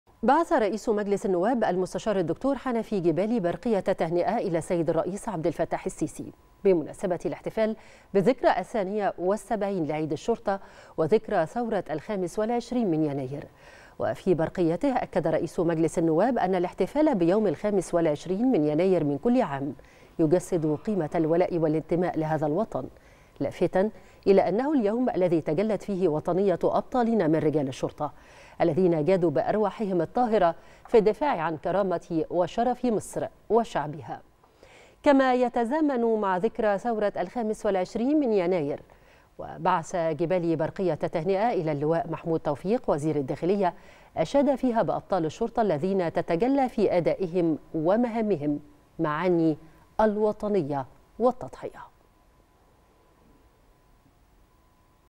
رئيس مجلس النواب يبعث برقية تهنئة إلى الرئيس السيسي بمناسبة الاحتفال بالذكرى 72 لعيد الشرطة و25يناير
نشرة الثالثة | رئيس مجلس النواب يبعث برقية تهنئة إلى الرئيس السيسي بمناسبة الاحتفال بالذكرى 72 لعيد الشرطة وذكرى ثورة 25 يناير
نشرة الثالثة